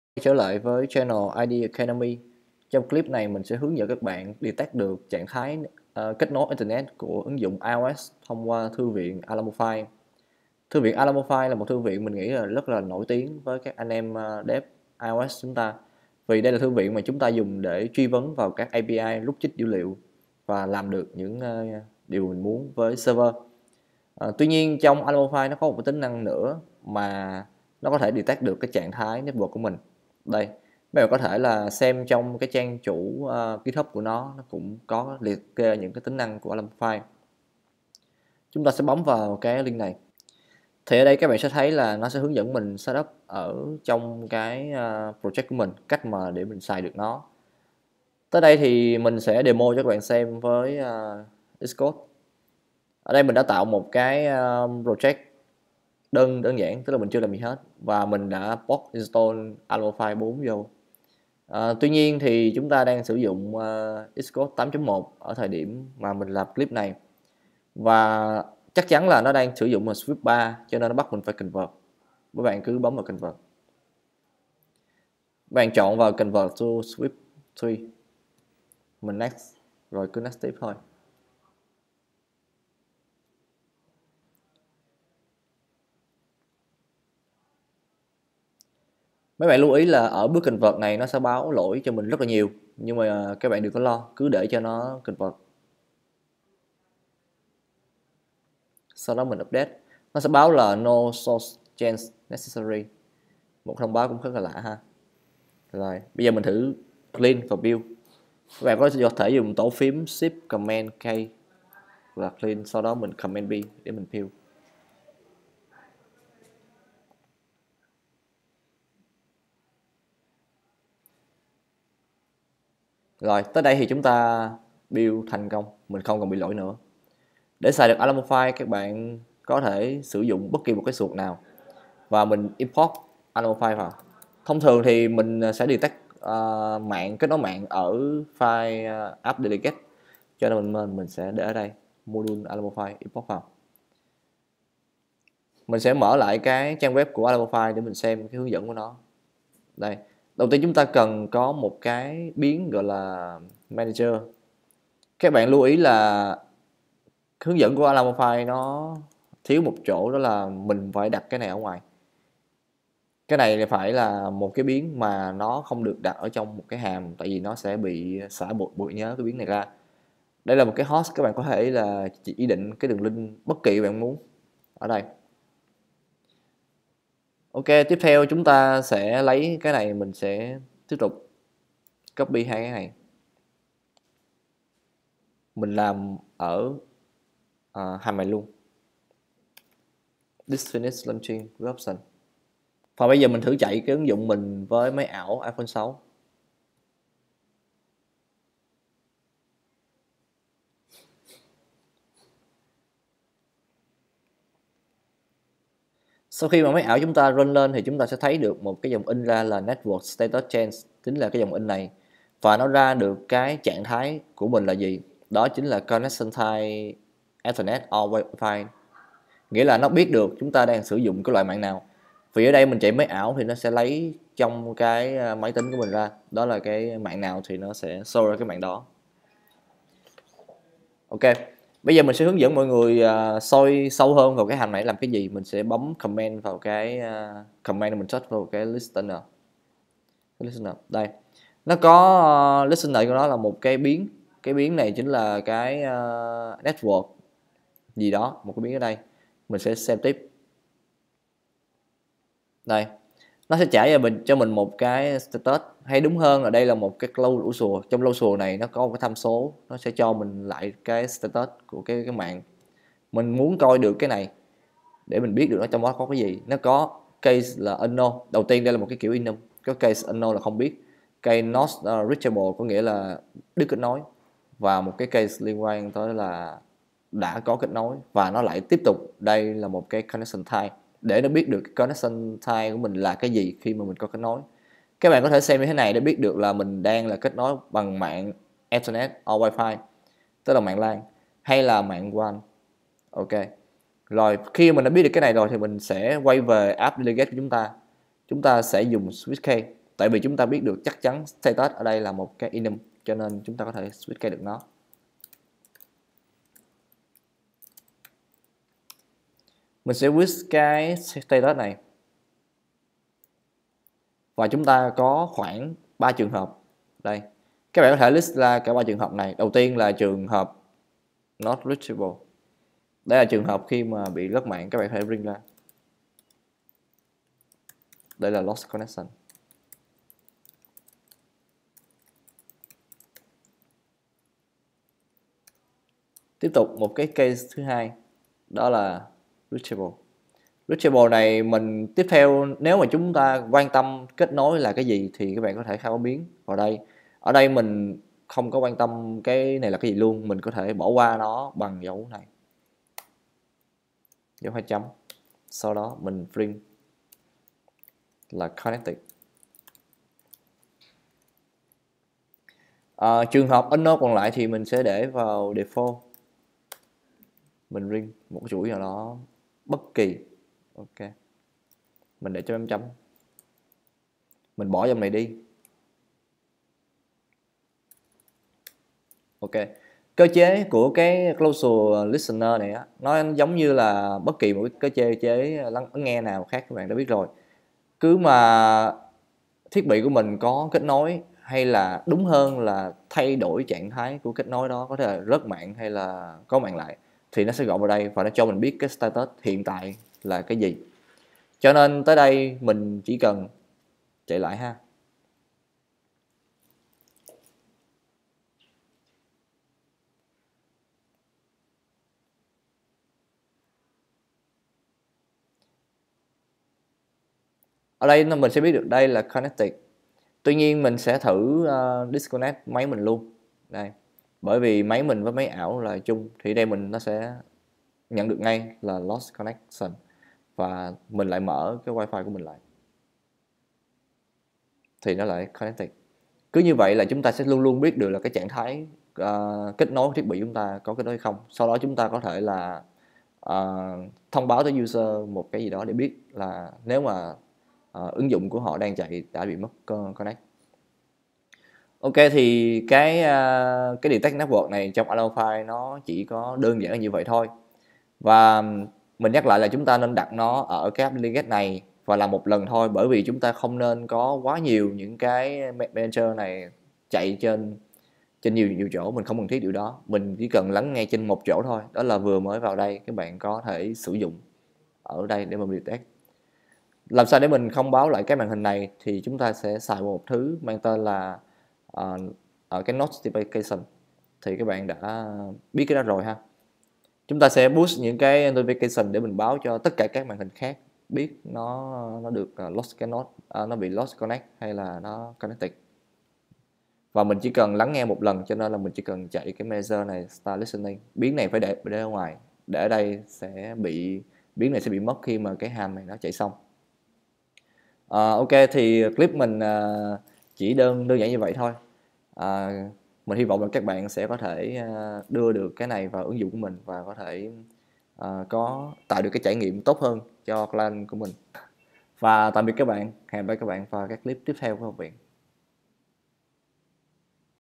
Detect network với Alamofire v4.x
Alamofire là một thư viện rất nổi tiếng trong cộng đồng lập trình iOS, nó được sử dụng đa số là để kết nối tới API, request data từ server...Tuy nhiên có một chức năng rất hay mà thường người ta không để ý tới đó chính là Network Reachability, kiểm tra được ứng dụng có đang kết nối mạng hay không?